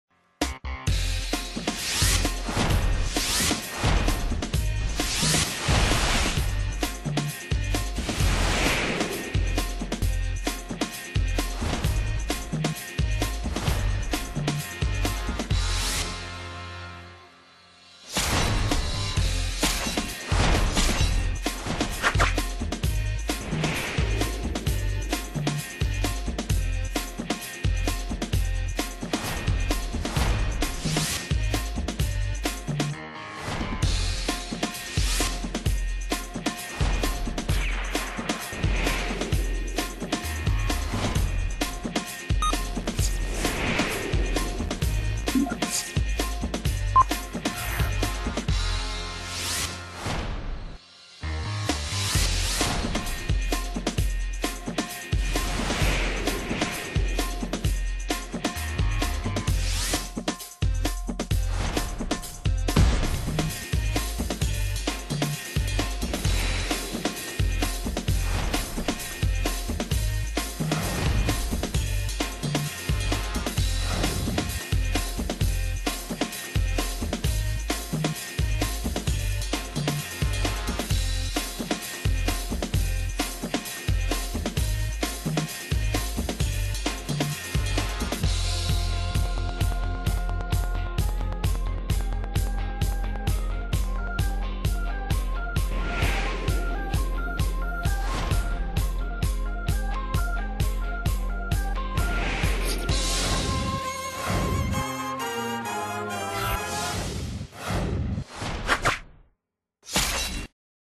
FOX13 Graphics Composite
This was a Midsouth Region Emmy Nominated Graphics montage